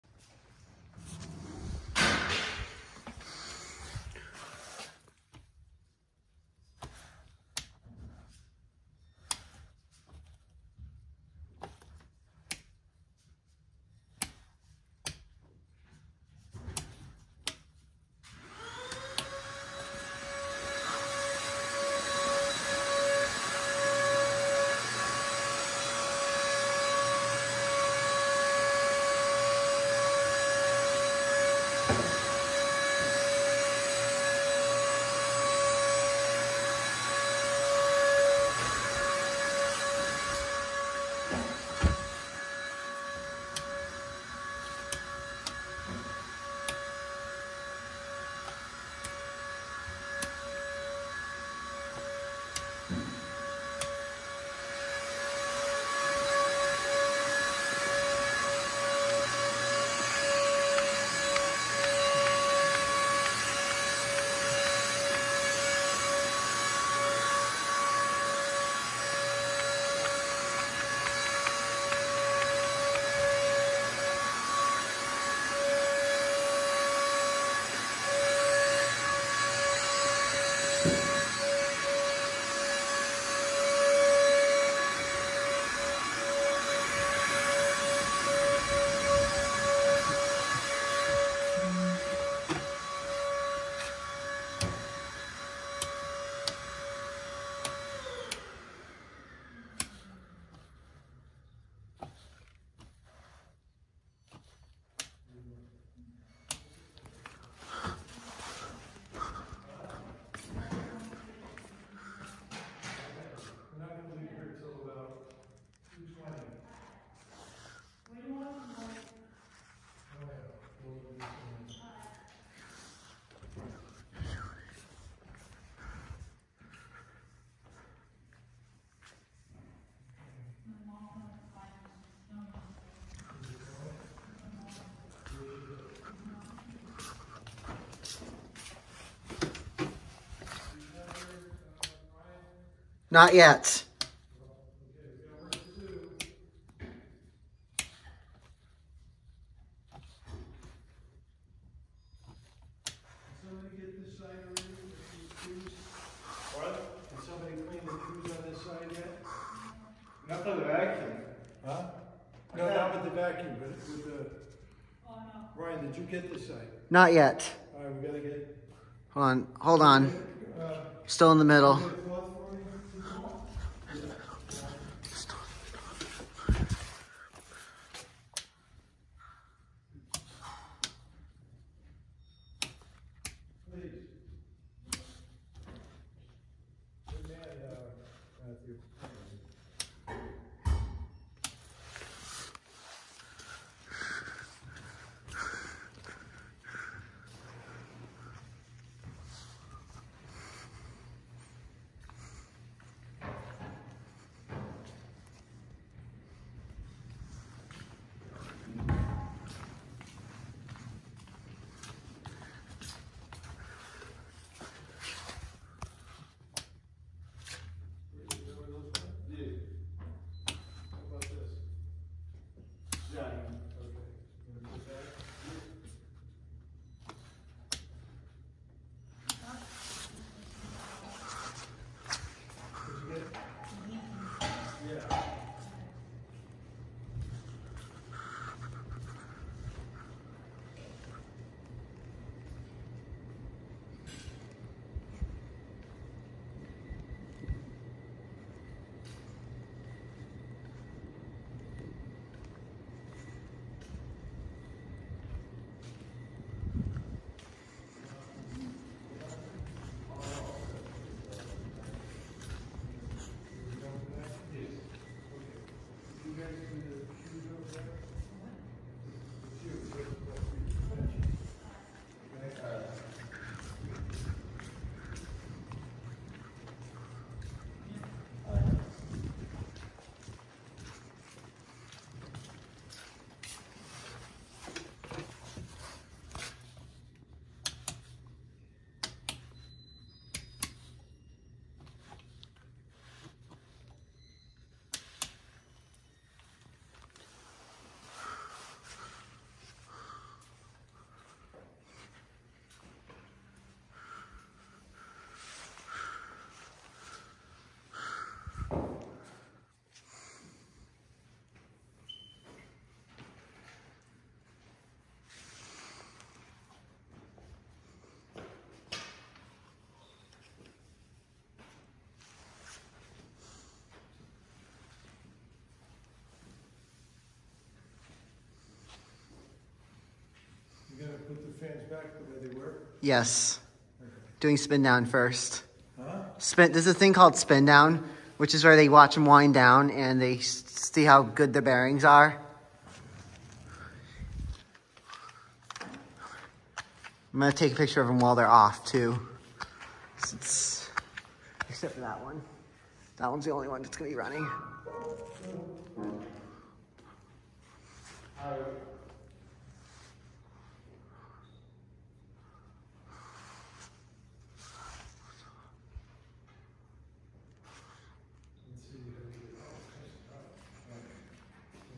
Casablanca Zephyr Ceiling Fans 2-7 of 7.
Here are the other Casablanca Zephyrs in the church I intern at. Afew of these fans have a higher pitched piezos then fan 2 and the one in the transcript which mightve been a replacement model because the others have the fat switch housing and the RMM1 board except for fan 2 which has a replacement RMM3 board. The one in the transcript also only works in reverse because I tried putting it in forward and it didn't do the hi-lo tone for reverse and it has a RMM2 board, it also might be a 4/5 because of the skinnier switch housing and it uses a W32 control.
Fan#4 has a broken piezo because it doesn't sound when you change the speed and it also uses a W32 switch due to the W11 switch going bad.
exscuse the loud vaccuum at 0:18, my friend was vaccuming the church.